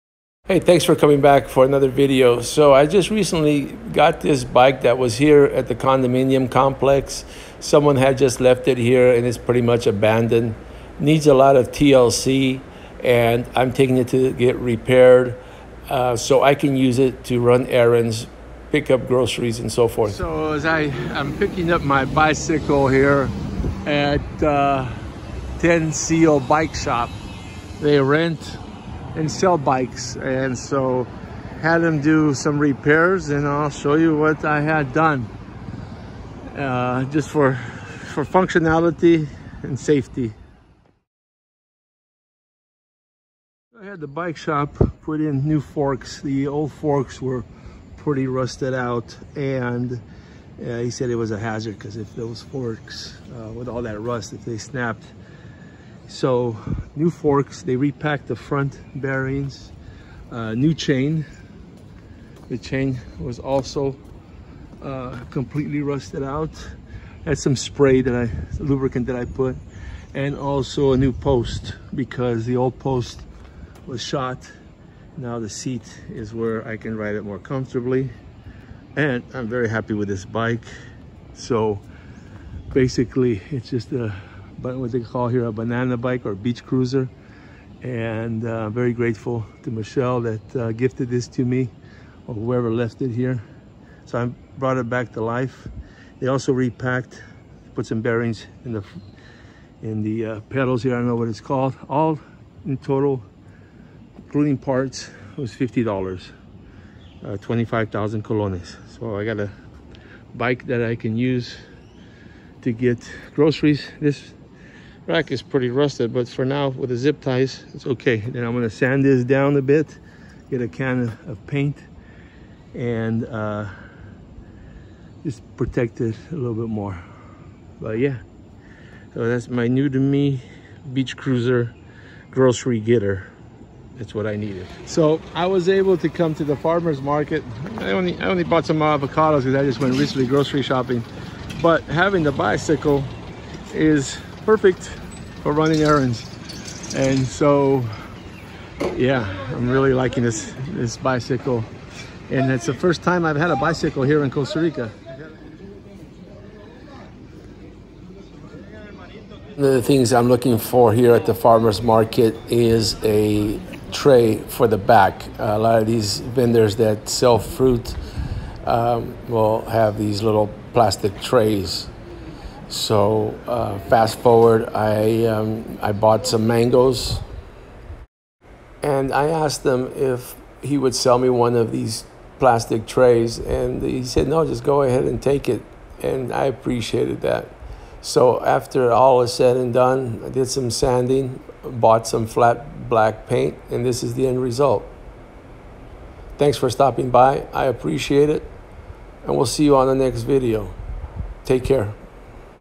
New to me Beach Cruiser Bicycle
Abandoned old rusty bicycle brought back to good working condition